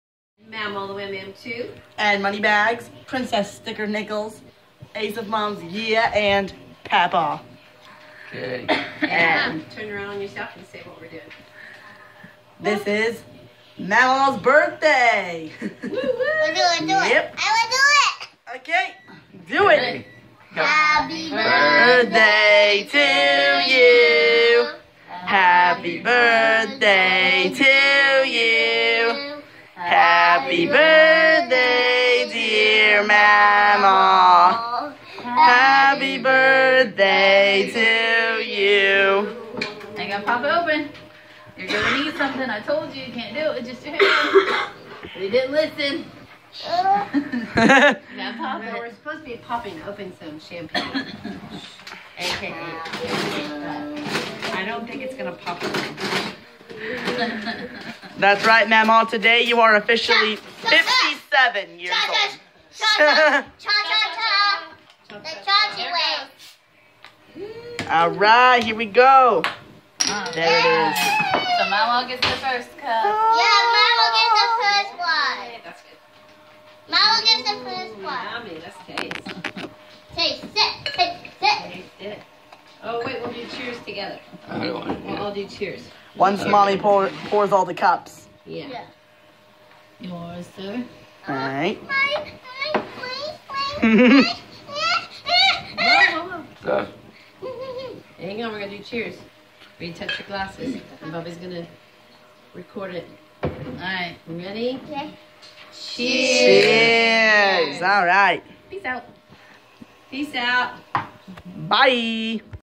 Mamaw TheWamam2's birthday celebration 2019!
That's right, she's officially...57 years old.